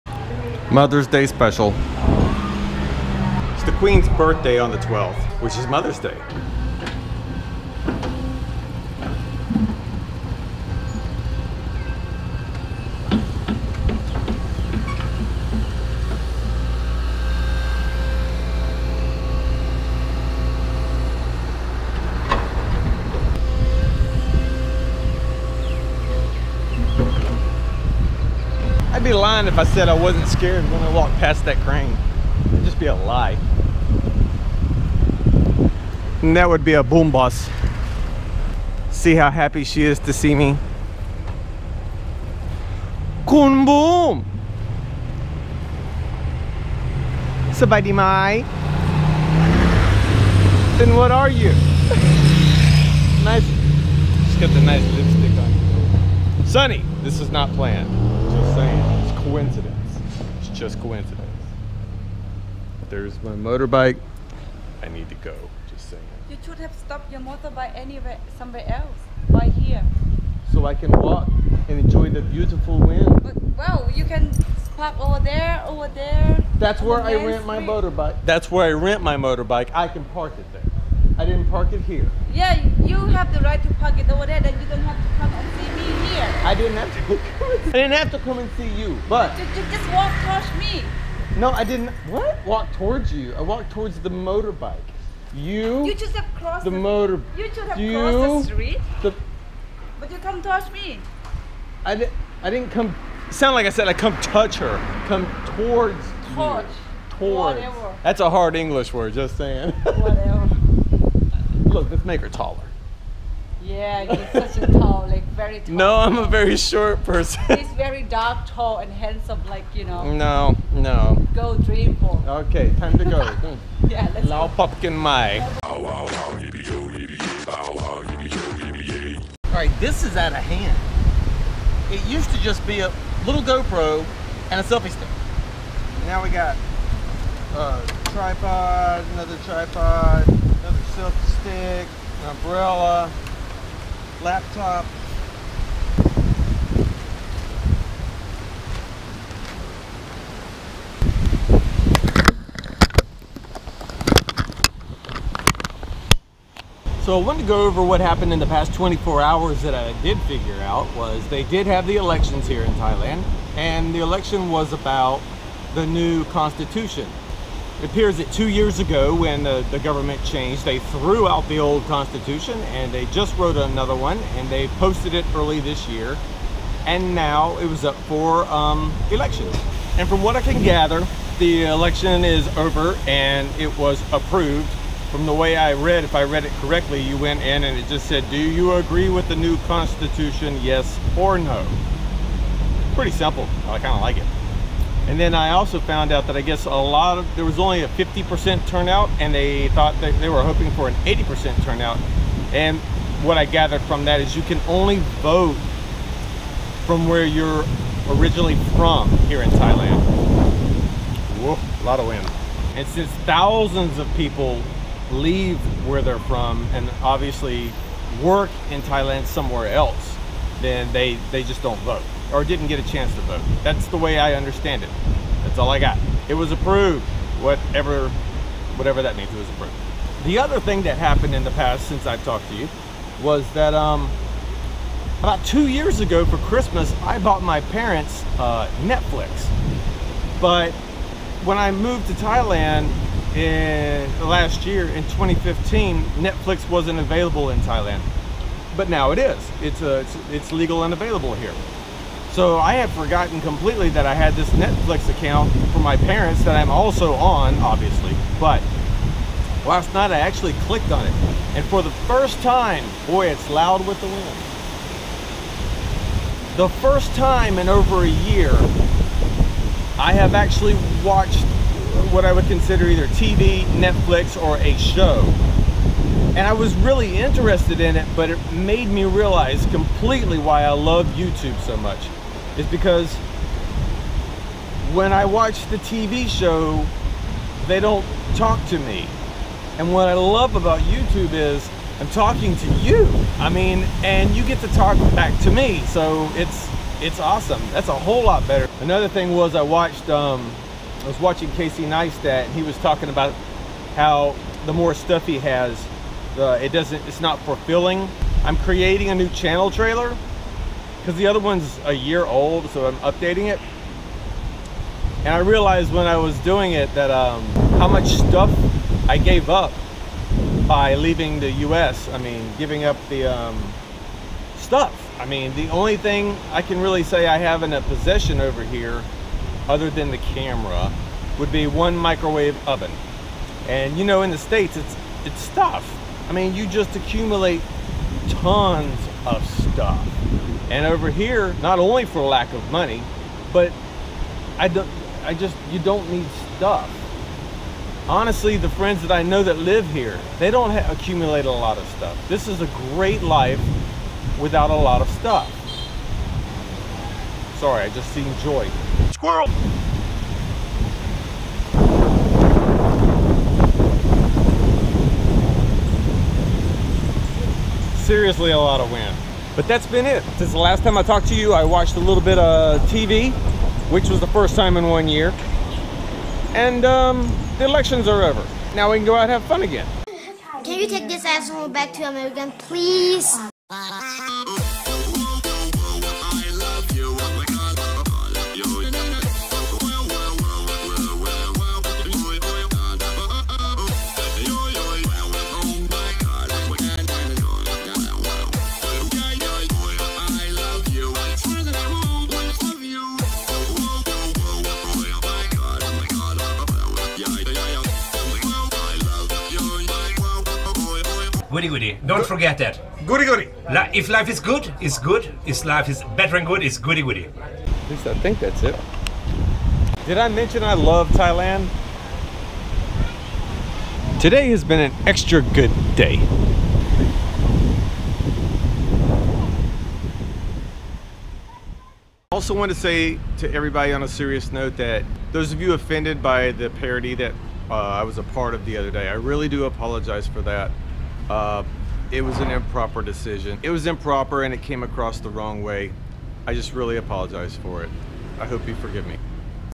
GOVERNMENT ELECTION / NETFLIX NOW LEGAL V159
Thailand had an election and Netflix is now legal here.

Shipping Address.
The address to ship to is:
Thailand Rob
C/O White Sun Bar
121 Moo 3, Beach Rd